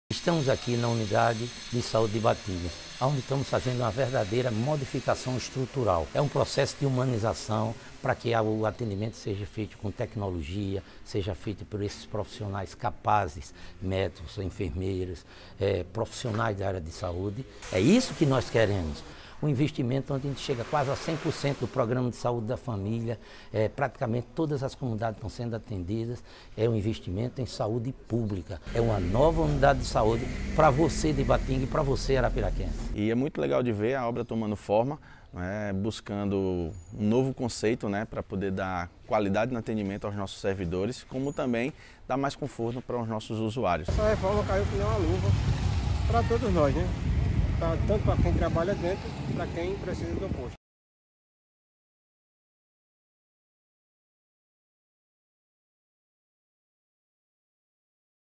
REFORMA NA UBS BATINGAS - ARAPIRACA
A espera acabou.
Estamos trabalhando para melhorar a saúde de Arapiraca com reformas nas unidades, incentivando programas como o Cria Mais Perto de Você e o Saúde na Hora, além da chegada do Hospital de Amor e a UPA. 😍🙌🏻⠀
Já são oito unidades sendo reformadas simultaneamente. O Centro de Saúde do bairro Batingas é um deles e, após concluído, vai atender milhares de pessoas com uma nova estrutura. 👩🏽‍⚕⠀
Dê o play e confira! ▶